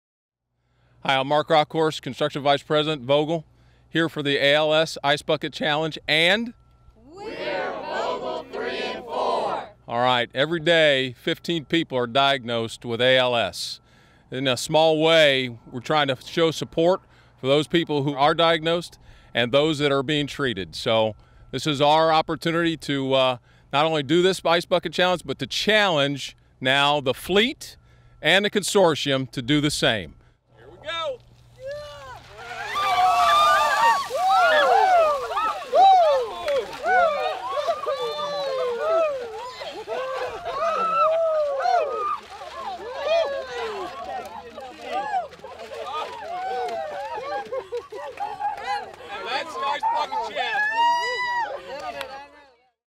Vogtle 3 and 4 employees take the Ice Bucket Challenge
Vogtle 3 and 4 Vice President of Construction Mark Rauckhorst and a group of Vogtle 3 and 4 team members took the ice bucket challenge with a twist – they used their hardhats as the bucket! “Everyday 15 people are diagnosed with ALS. In a small way, we’re trying to show support for those people who are diagnosed and those that are being treated,” said Rauckhorst. He went on to challenge the construction consortium of CB&I and Westinghouse as well as the Southern Nuclear Fleet to complete the Ice Bucket Challenge for ALS.